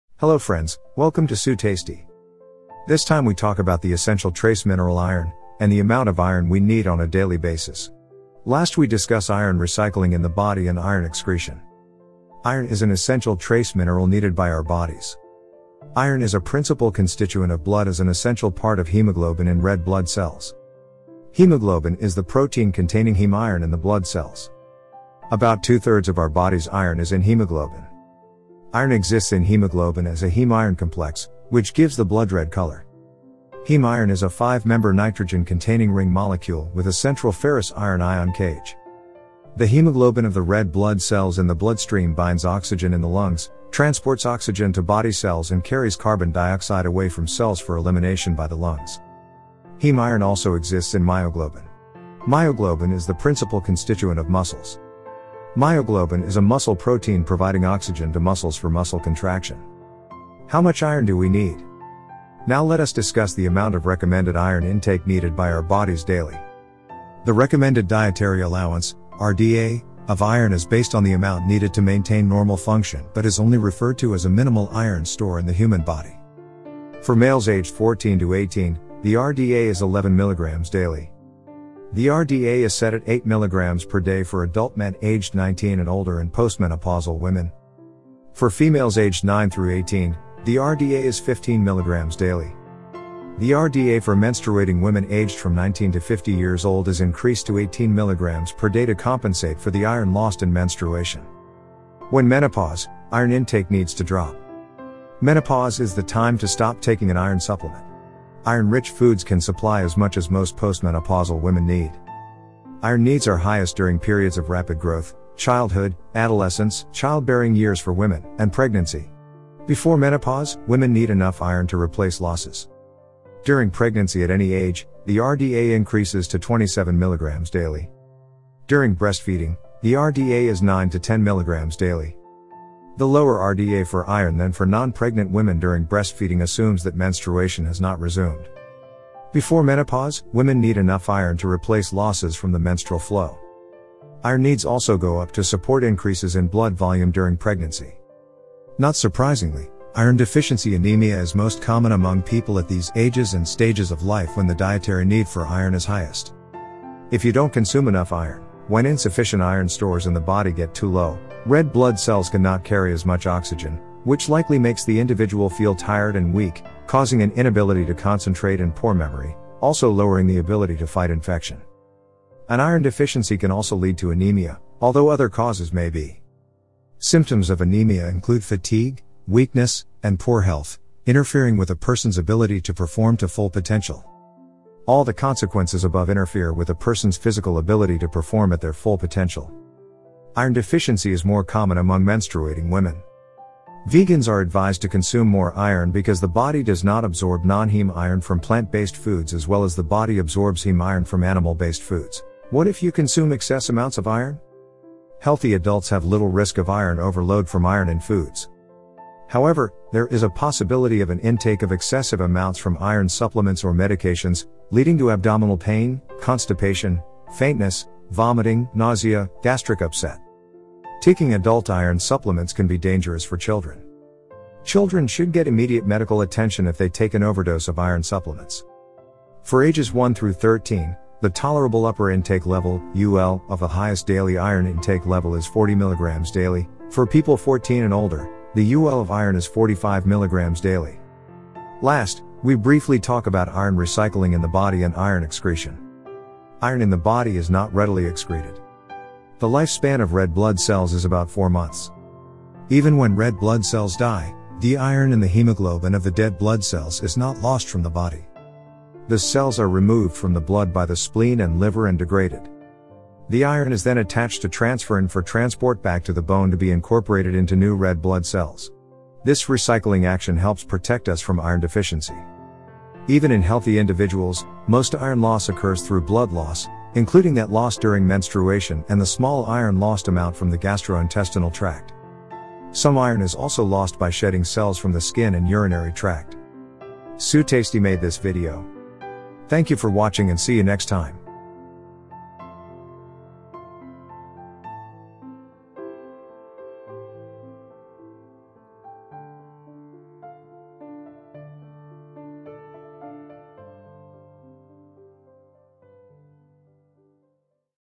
Iron (Fe) | the Essential Trace Mineral || How Much Iron Do We Need Daily?
Content index:
00:11 Iron is an essential trace mineral needed by our bodies.
00:59 How much iron do we need?
02:30 What if you don’t consume enough iron.
03:21 What if you consume excess amounts of iron?
04:01 Iron recycling in the body and Iron excretion.

The recommended dietary allowance, RDA, of iron is based on the amount needed to maintain normal function but is only referred to as a minimal iron store in the human body.
For males aged 14 to 18, the RDA is 11 milligrams daily.
The RDA is set at 8 milligrams per day for adult men aged 19 and older and post-menopausal women.
For females aged 9 through 18, the RDA is 15 milligrams daily.
The RDA for menstruating women aged from 19 to 50 years old is increased to 18 milligrams per day to compensate for the iron lost in menstruation.
When menopause, iron intake needs to drop.
Menopause is the time to stop taking an iron supplement. Iron-rich foods can supply as much as most post-menopausal women need.
Iron needs are highest during periods of rapid growth: childhood, adolescence, child-bearing years for women, and pregnancy. Before menopause, women need enough iron to replace losses.

During pregnancy at any age, the RDA increases to 27 milligrams daily.
During breastfeeding, the RDA is 9  to 10 milligrams daily.
The lower RDA for iron than for non-pregnant women during breastfeeding assumes that menstruation has not resumed.

Before menopause, women need enough iron to replace losses from the menstrual flow.
Iron needs also go up to support increases in blood volume during pregnancy.
Not surprisingly, iron deficiency anemia is most common among people at these ages and stages of life when the dietary need for iron is highest.

Iron deficiency is more common among menstruating women.
Vegans are advised to consume more iron because the body does not absorb nonheme iron from plant-based foods as well as the body absorbs heme iron from animal-based foods.

However, there is a possibility of an intake of excessive amounts from iron supplements or medications, leading to abdominal pain, constipation, faintness, vomiting, nausea, gastric upset.

Taking adult iron supplements can be dangerous for children.
Children should get immediate medical attention if they take an overdose of iron supplements.

For ages one through 13, The Tolerable Upper Intake Level, UL,  of the highest daily iron intake level is 40 milligrams daily; for people 14 and older, the UL of iron is 45 milligrams daily.

Iron recycling in the body and Iron excretion.
Iron in the body is not readily excreted.
The life span of red blood cells is about four months.
Even when red blood cells die, the iron in the hemoglobin of the dead blood cells is not lost from the body.
The cells are removed from the blood by the spleen and liver and degraded.
The iron is then attached to transferrin for transport back to the bone to be incorporated into new red blood cells. This recycling action helps protect us from iron deficiency.

Even in healthy individuals, most iron loss occurs through blood loss, including that lost during menstruation and the small iron lost amount from the gastrointestinal tract.
Some iron is also lost by shedding cells from the skin and urinary tract.

SuTasty made this video.